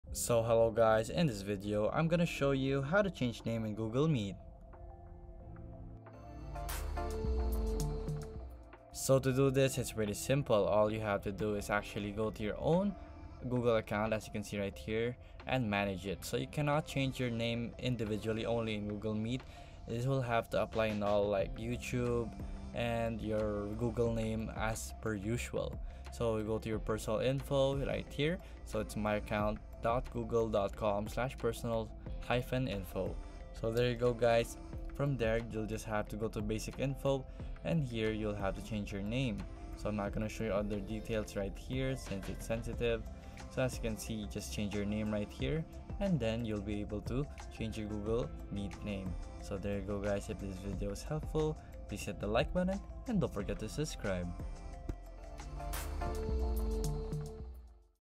How To Change Name In Google Meet | Quick Tutorial (2025)
👉 In this quick tutorial video, we'll show you how to easily change your name in Google Meet. Whether you're using the app or accessing it through your browser, we'll guide you through the steps to change your name on Google Meet. By the end of this video, you'll know how to change your name in Google Meet and make sure you're presenting yourself accurately in your virtual meetings.